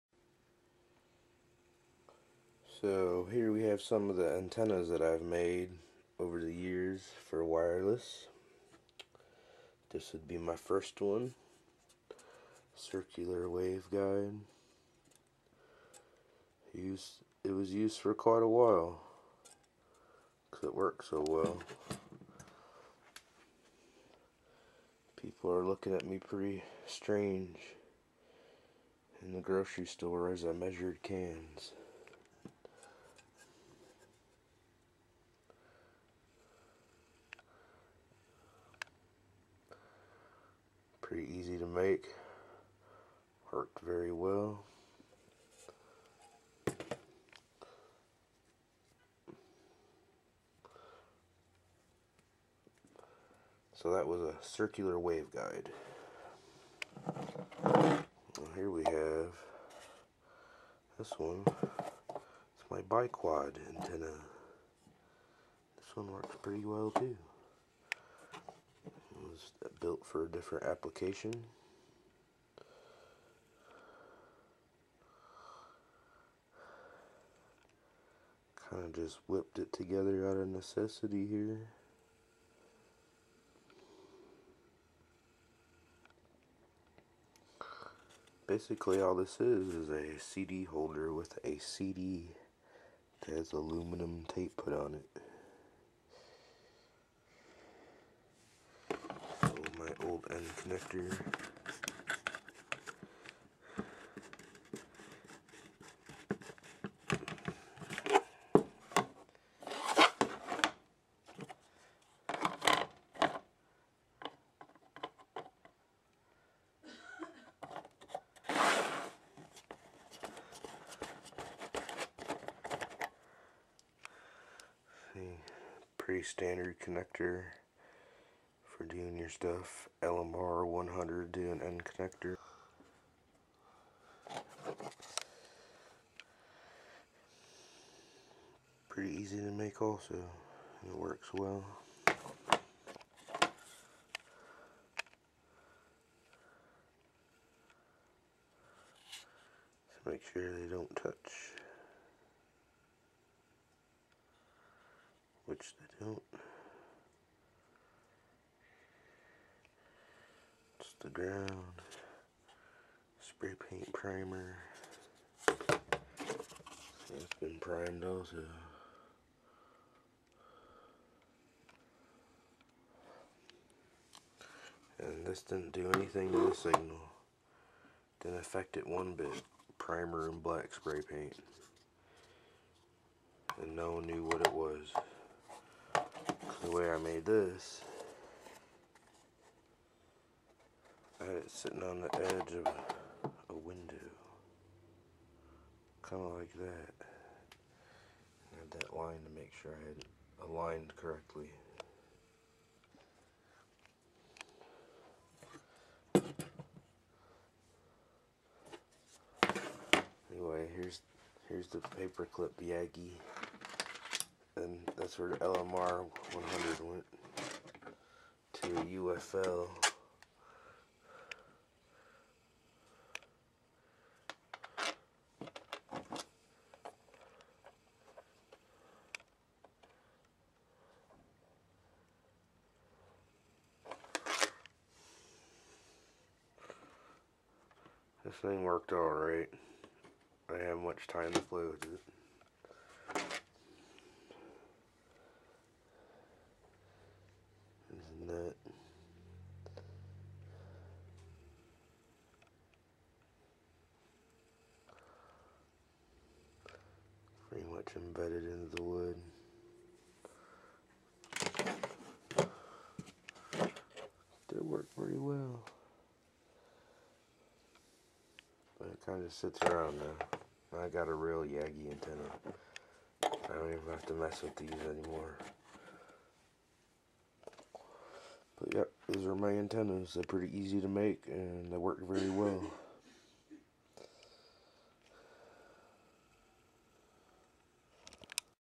A few wifi antenna
Here are a few antenna  Ive made and used over the years. Circular Waveguide(cantenna), Bi-Quad, Yagi.